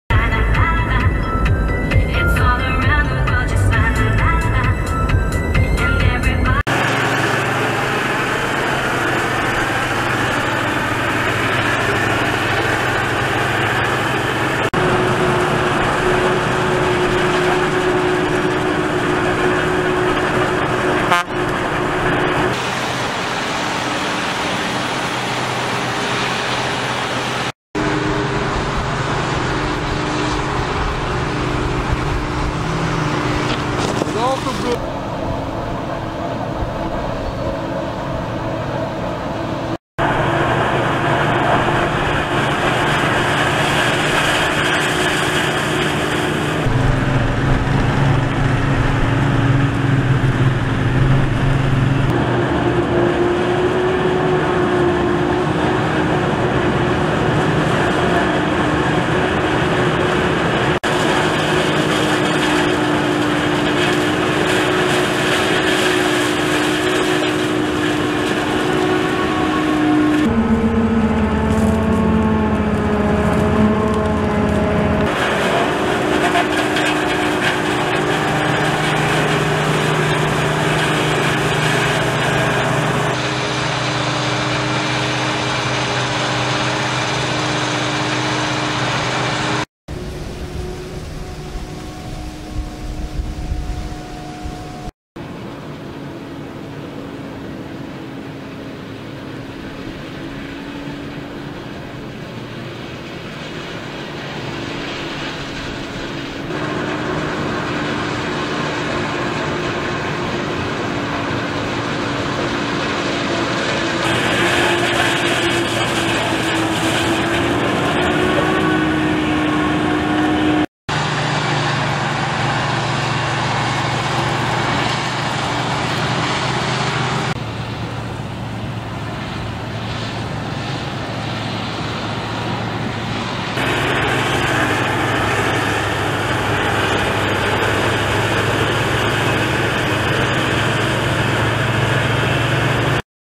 Krone Big X780 mit 5 Gespanne im Mais!
Mais Ernte bei Kleve, NRW im September 2021.